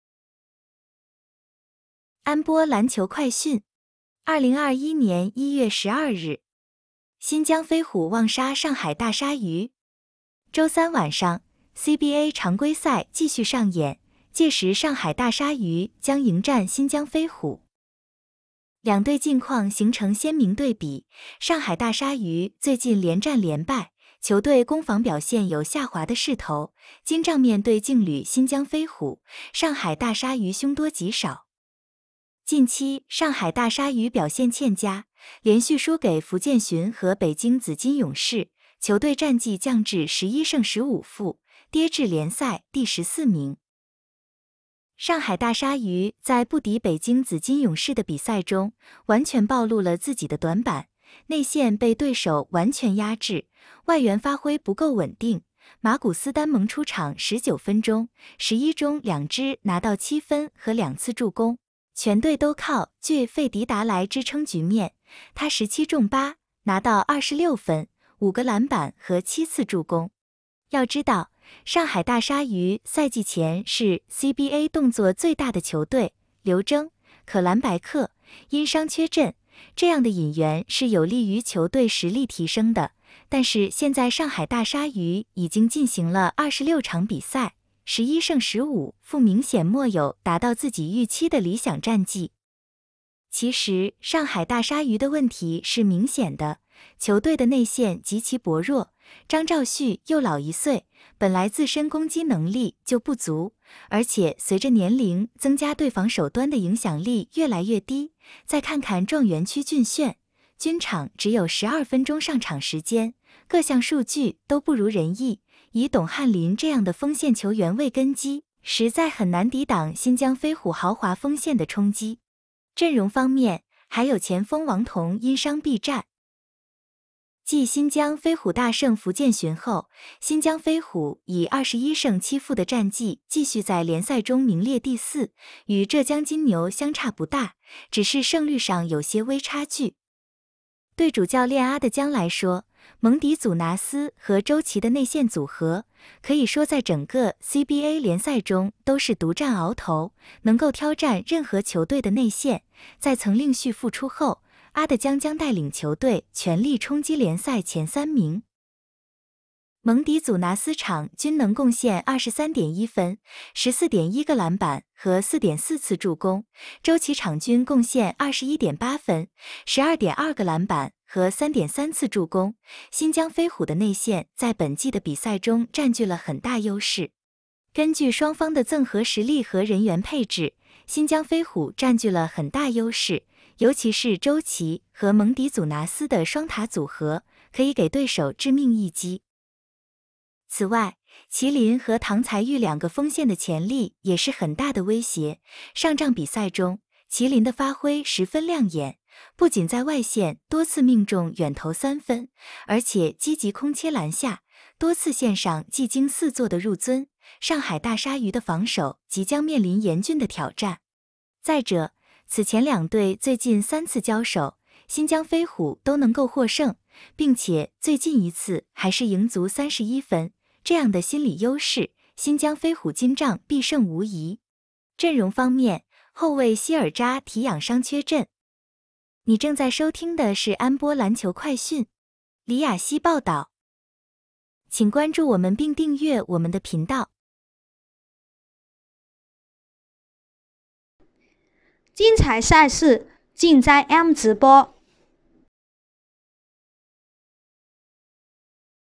(普)【CBA-賽前新聞】2021-01-13 上海大鯊魚 VS 新疆飛虎 | 新疆飛虎望殺上海大鯊魚
週三晚上，CBA 常規賽繼續上演，屆時#上海大鯊魚 將迎戰#新疆飛虎 。兩隊近況形成鮮明對比，上海大鯊魚最近連戰連敗，球隊攻防表現有下滑的勢頭，今仗面對勁旅新疆飛虎，上海大鯊魚凶多吉少。

近期上海大鯊魚表現欠佳，連續輸給福建鱘和北京紫禁勇士，球隊戰績降至11勝15負，跌至聯賽第14名。上海大鯊魚在不敵北京紫禁勇士的比賽中，完全暴露了自己的短板，內線被對手完全壓制，外援發揮不夠穩定，馬古斯丹蒙出場19分鐘，11中2只拿到7分和2次助攻，全隊都靠J.費迪達來支撐局面，他17中8，拿到26分、5個籃板和7次助攻。要知道，上海大鯊魚賽季前是CBA動作最大的球隊，劉錚、可蘭白克(因傷缺陣)這樣的引援是有利於球隊實力提升的，但是現在上海大鯊魚已經進行了26場比賽，11勝15負明顯沒有達到自己預期的理想戰績。其實上海大鯊魚的問題是明顯的，球隊的內線極其薄弱，張兆旭又老一歲，本來自身攻擊能力就不足，而且隨著年齡增加對防守端的影響力越來越低，再看看狀元區俊炫，均場只有12分鐘上場時間，各項數據都不如人意，以董瀚麟這樣的鋒線球員為根基，實在很難抵擋新疆飛虎豪華鋒線的衝擊。陣容方面，還有前鋒王潼因傷避戰。

繼新疆飛虎大勝福建鱘後，新疆飛虎以21勝7負的戰績繼續在聯賽中名列第四，與浙江金牛相差不大，只是勝率上有些微差距。對主教練阿的江來說，蒙迪祖拿斯和周琦的內線組合，可以說在整個CBA聯賽中都是獨佔鰲頭，能夠挑戰任何球隊的內線，在曾令旭復出後，阿的江將帶領球隊全力衝擊聯賽前三名。蒙迪祖拿斯場均能貢獻23.1分、14.1個籃板和4.4次助攻，周琦場均貢獻21.8分、12.2個籃板和3.3次助攻，新疆飛虎的內線在本季的比賽中佔據了很大優勢。根據雙方的綜合實力和人員配置，新疆飛虎佔據了很大優勢，尤其是周琦和蒙迪祖拿斯的雙塔組合，可以給對手致命一擊。此外，齊麟和唐才育兩個鋒線的潛力也是很大的威脅，上仗比賽中，齊麟的發揮十分亮眼，不僅在外線多次命中遠投三分，而且積極空切籃下，多次獻上技驚四座的入樽，上海大鯊魚的防守即將面臨嚴峻的挑戰。再者，此前兩隊最近3次交手，新疆飛虎都能夠獲勝，並且最近一次還是贏足31分，這樣的心理優勢，新疆飛虎今仗必勝無疑。陣容方面，後衛西爾紮提養傷缺陣。